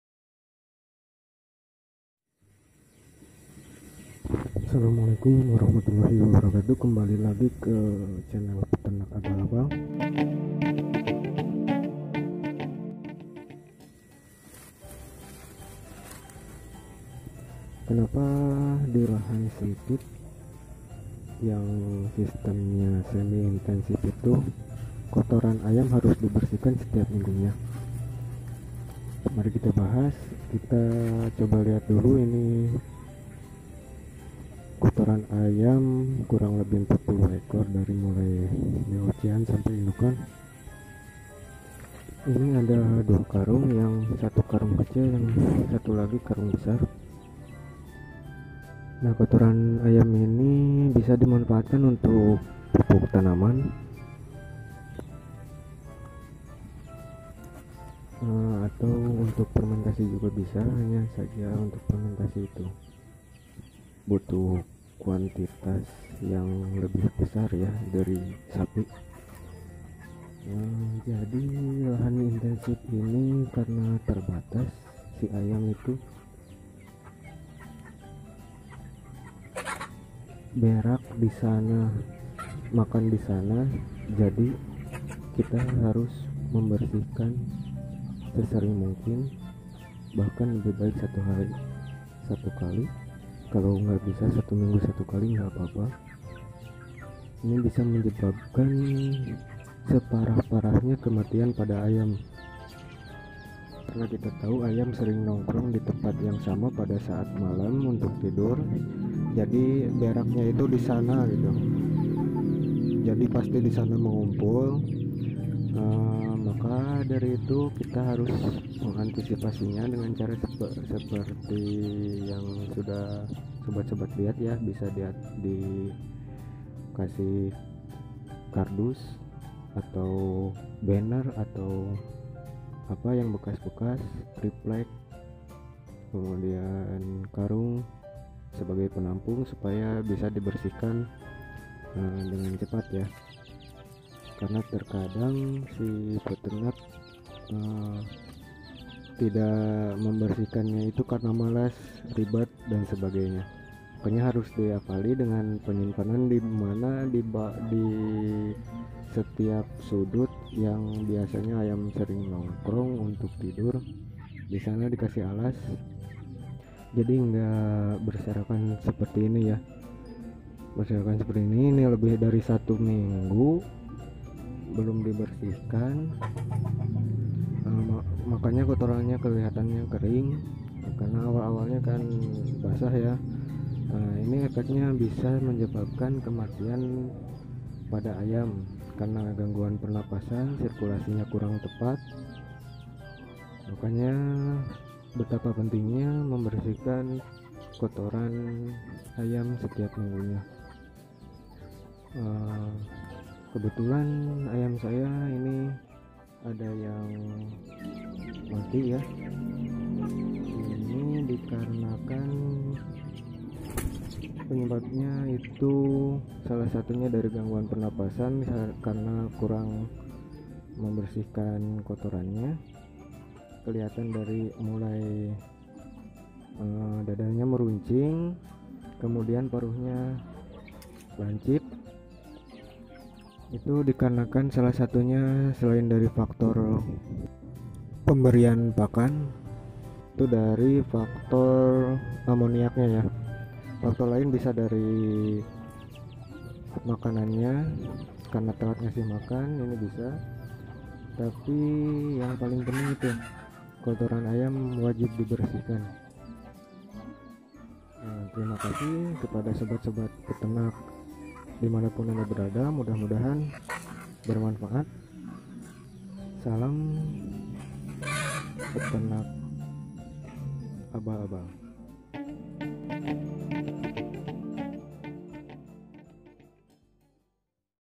Kenapa harus membersihkan kotoran ayam di lahan sempit sistem semi intensif
Mohon maaf, tanpa mengurangi rasa hormat pada sesepuh ahli peternakan khususnya dalam hal management kandang, saya meng-upload video tentang kotoran ayam.

Menurut pengalaman saya PeternakAbalAbal , sering sekali ayam sakit ngorok, dada lancip, paruh meruncing, kaki kurus dikarenakan salahsatu faktor dari kotoran ayam yang menimbulkan amoniak tinggi.
Ini perlu diperhatikan pada saat DOC karena saat itulah masa "krusial" bagi pertumbuhan dan kekebalan dilihat dari sisi eksternal (faktor luar)

Silahkan KRITIK jika tak tepat, SHARE jika bermanfaat.
"Jika ilmu disembunyikan, tak perlu ada pendidikan"